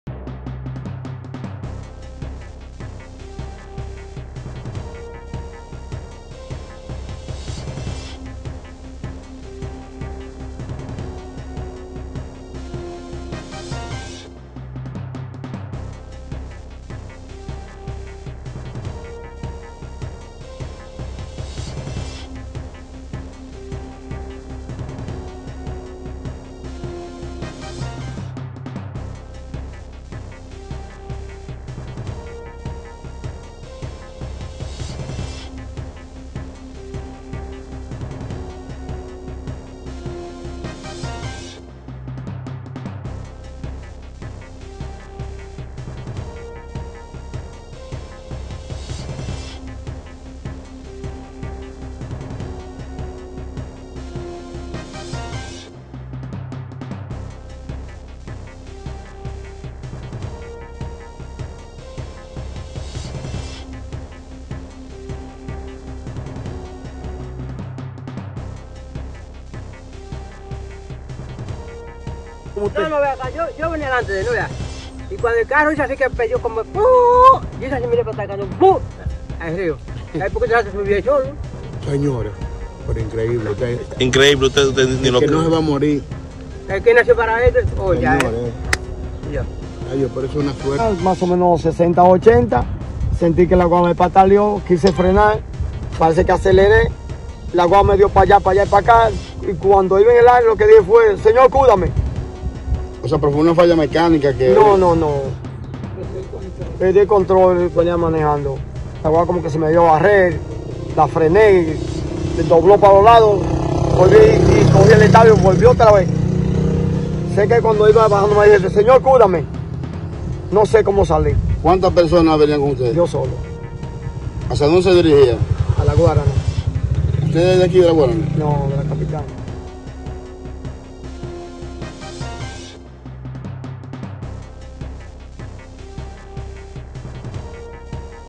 ¡Vivo de milagro! Conductor resultó con heridas leves tras caer de un puente al rio Camú
San Francisco De Macorís.- Un hombre salió ileso luego de que perdiera el control de su vehículo y cayera al río sobre el puente Camú, en la carretera entre los municipios de Cotuí y Las Guáranas.

El accidentando solo recibió heridas leves en la cara, mientras transitaba por la vía, y explicó que conducía en velocidad entre 60 y 80 kilómetros por hora, cuando perdió el control de la jeepeta marca Honda modelo CRV.

Personas residentes en las cercanías llegaron para ser socorrer al conductor, tras el hecho ocurrido en horas de la tarde del domingo 28 de mayo, y calificaron lo sucedido como “increíble”.